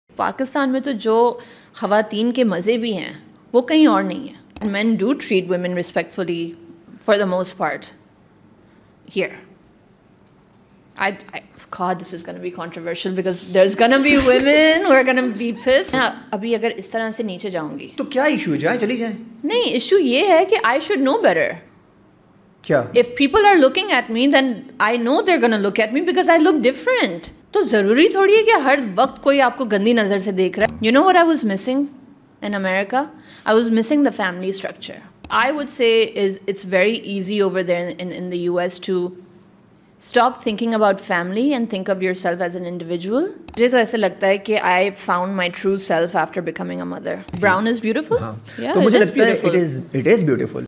Unfiltered Chat: Hira Tareen Opens Up | Adnan Faisal Podcast
Join us for an insightful conversation with Hira Tareen on the latest episode of the Adnan Faisal Podcast. Explore the depths of candid discussion as we delve into various aspects of life, career, and beyond. From personal anecdotes to professional insights, Hira Tareen shares her journey with honesty and authenticity. Tune in to gain valuable perspectives and engage in thought-provoking dialogue. Don't miss out on this opportunity to uncover the stories behind the persona.